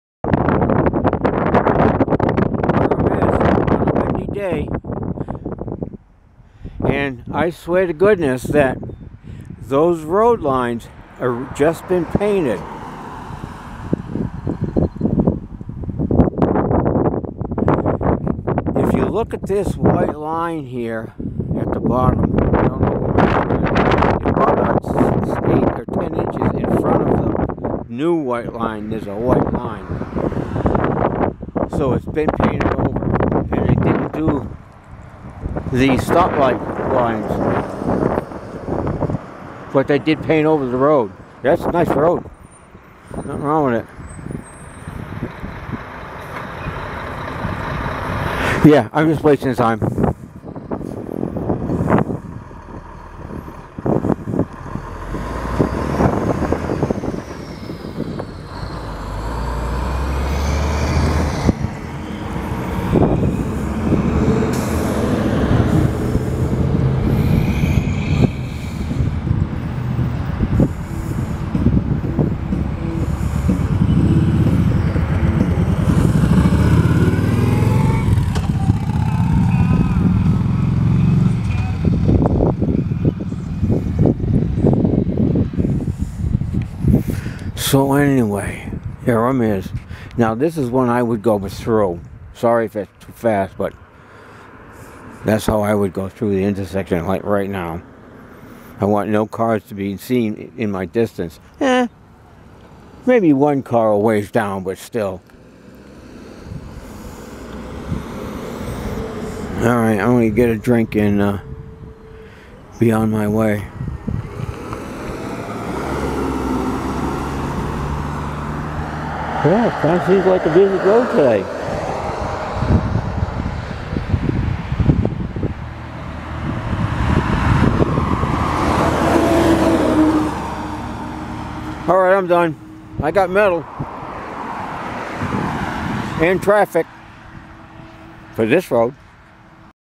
newly painted lines on route 205...
this state road has new orange and white painted lines...